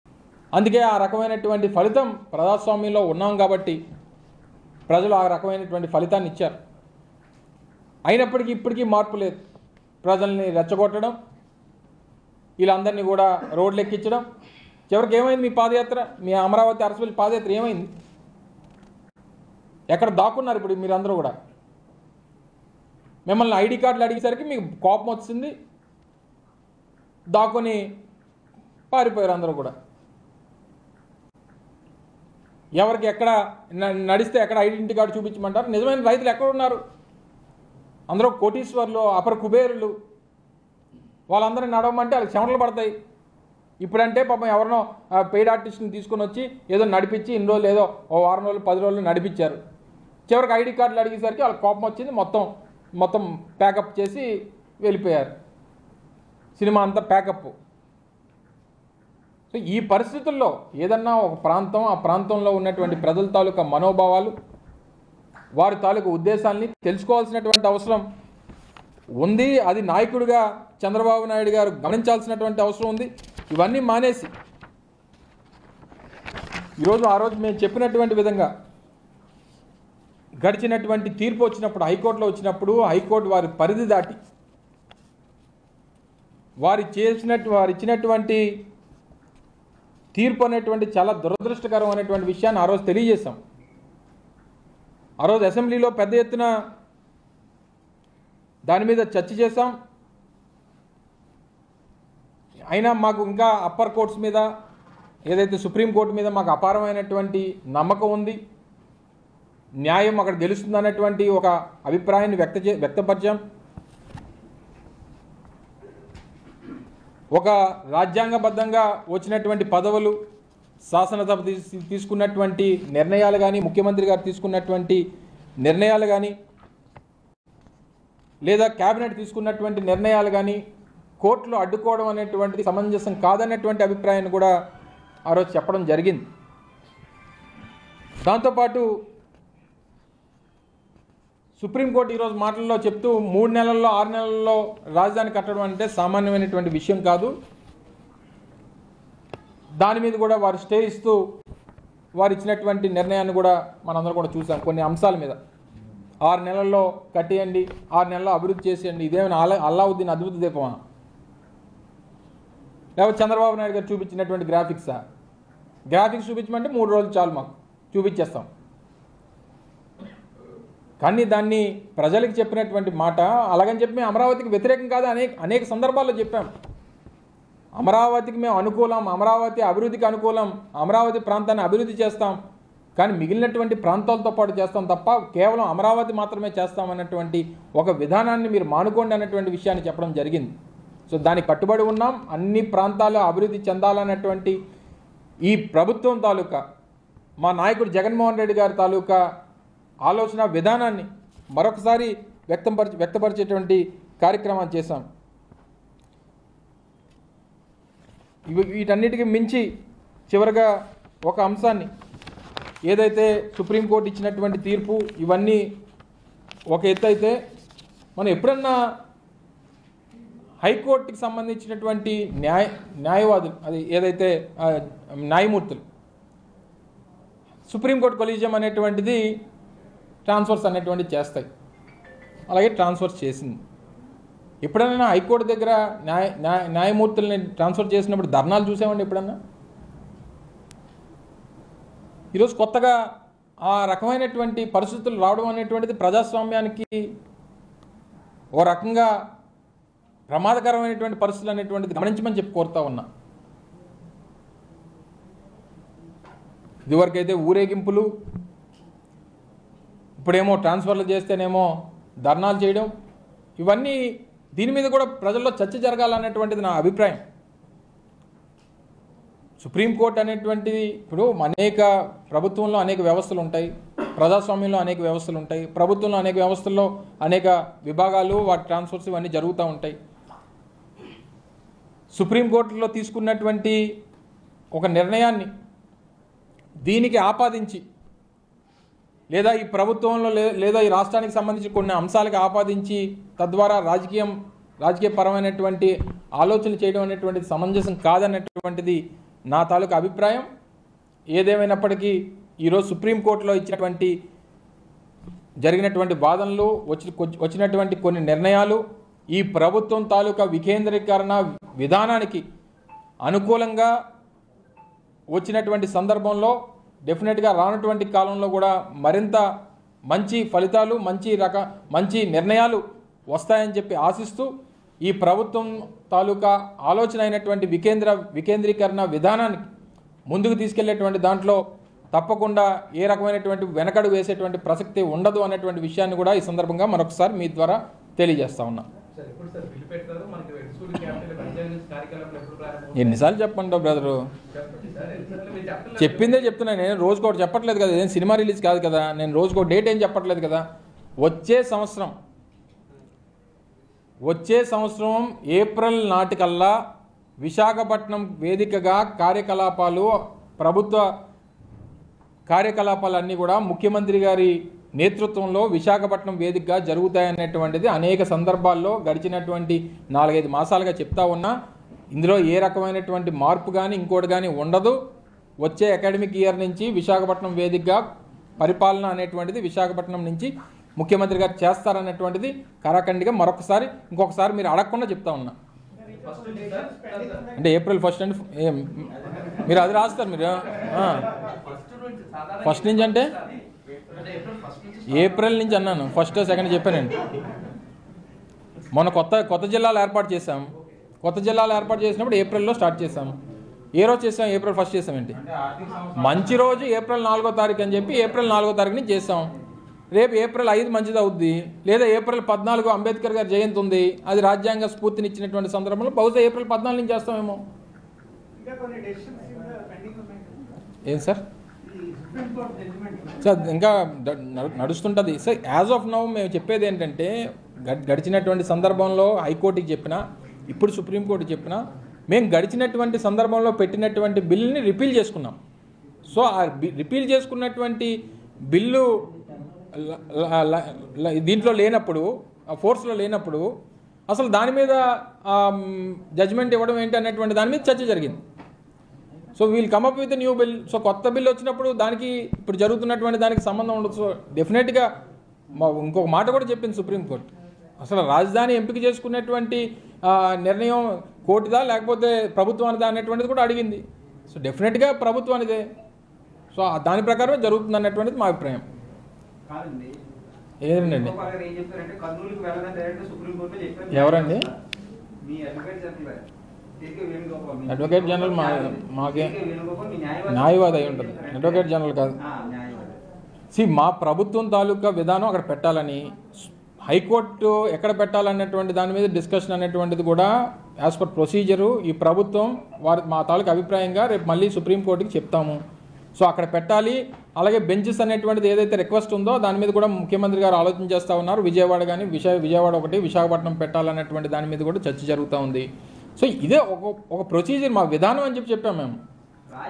Minister Gudivada Amarnath Satires on Amaravati Fake Farmers Padayatra
Minister Gudivada Amarnath Satires on Amaravati Fake Farmers Padayatra  @Sakshi TV Live